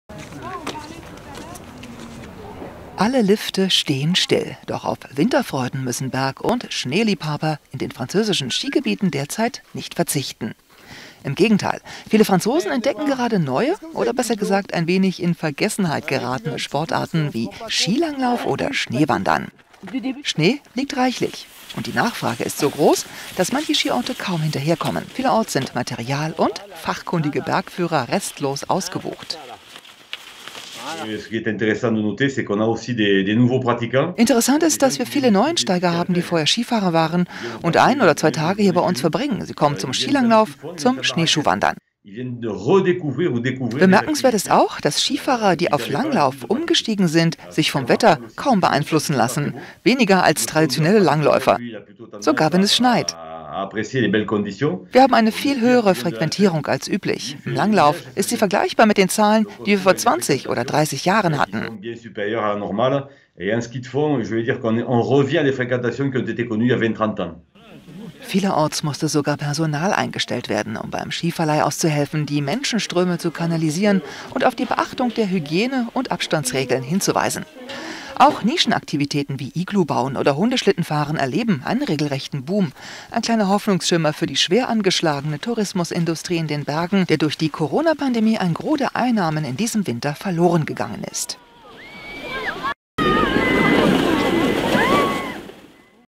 Langlauf und Iglubauen: Franzosen entdecken Winterurlaub mal anders
Viele Franzosen entdecken gerade neue, oder besser gesagt ein wenig Vergessenheit geratene Sportarten wie Skilanglauf oder Schneewandern. Die Nachfrage ist so groß, dass manche Skiorte kaum hinterherkommen.